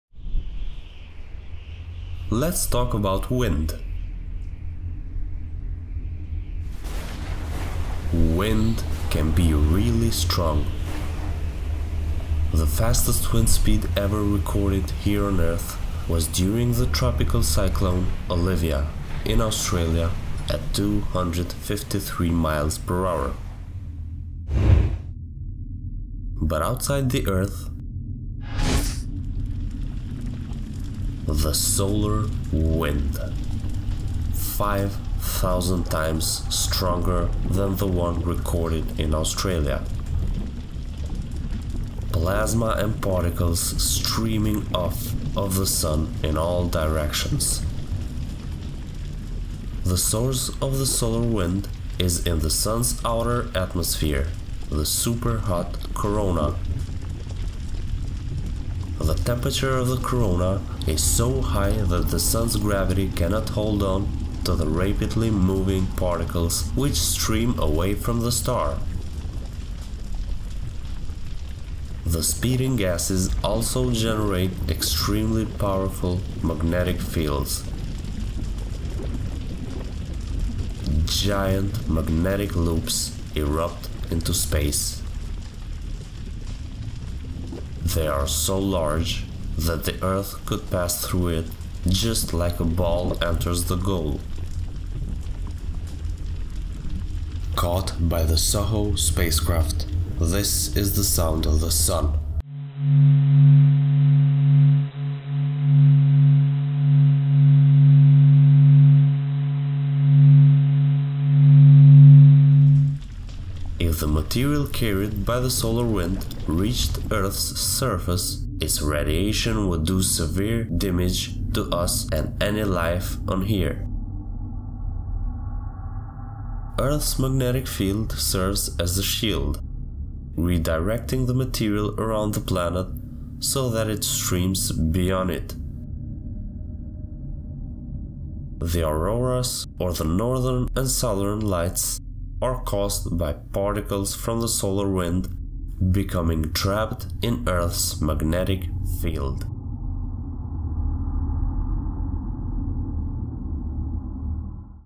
What is Solar Wind?
Have you ever wondered what is solar wind? The fastest wind speed ever recorded here on Earth was during the tropical cyclone Olivia in Australia at 253 mph. But in this video, we talk about the winds outside the Earth, the Solar Wind, 5000 times stronger than the wind recorded in Australia. During the video, you will also be able to listen to the sound of the Sun.

Graphics & credits:
Space Engine
NASA's Goddard Space Flight Center
NASA's Solar Dynamics Observatory (SDO)
NASA Goddard/Mary Pat Hrybyk-Keith
NASA Goddard’s Conceptual Image Lab/Josh Masters